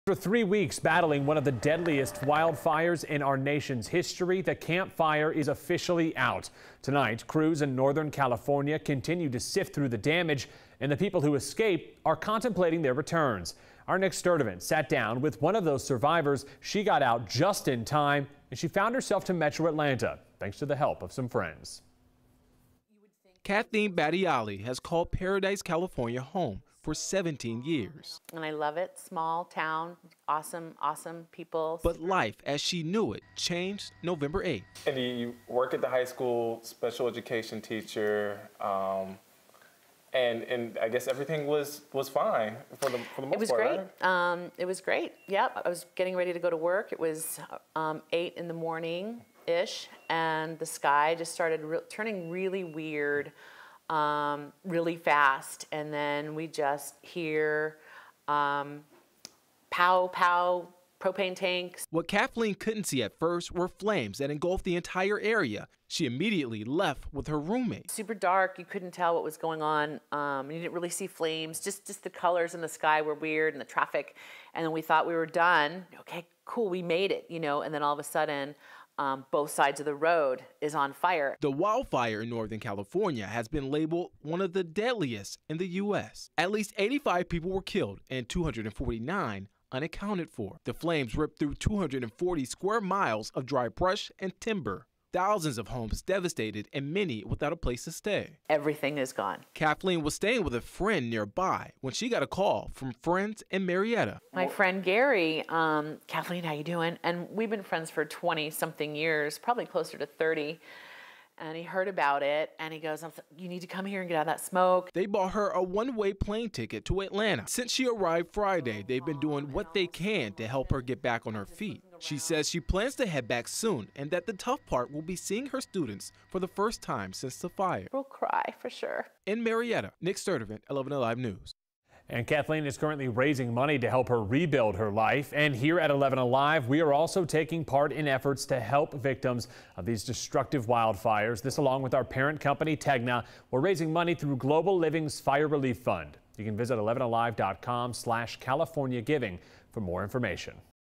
Camp fire survivor finds refuge in Metro Atlanta
One of those survivors who got out just in time found herself in Atlanta, thanks to the help of some friends.